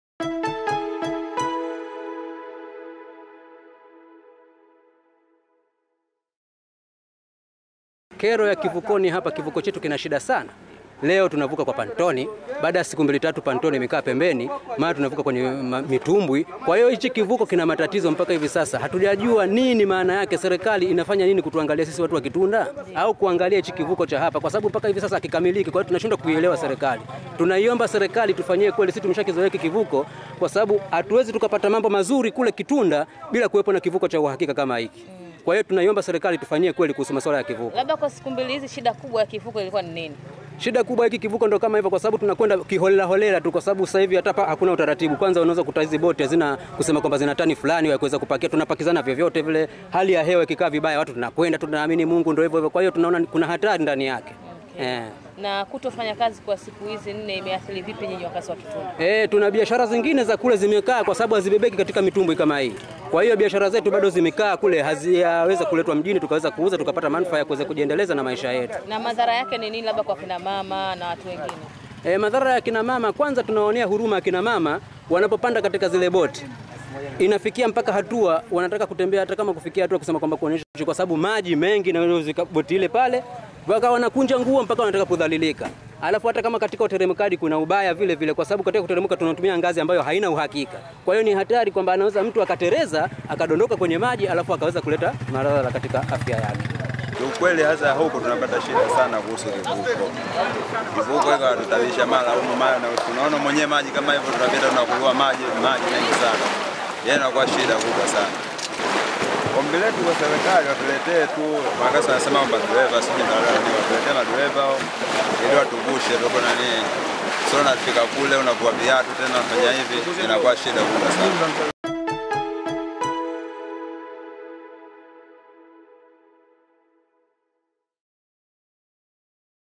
WAKAZI WA KITUNDA-LINDI WALIA NA KUSUASUA KWA HUDUMA YA KIVUKO
Wakazi wa Mtaa wa Kitumbikwela Manispaa ya Lindi,
wamelalamikia kusuasua kwa ufanyaji kazi wa Kivuko cha MV-
KITUNDA hali walioitaja kuhatarisha usalama wao.
Wakizungumza na Mashujaa TV, baadhi ya wakazi hao
wamesema hawaelewi tatizo lilipo kwani mara kwa mara kivuko
hicho kimekua hakifanyi kazi kwa uhakika na kusababisha
wananchi hao kuendelea kutumia Mitumbwi ambayo kwao ni
adha kubwa.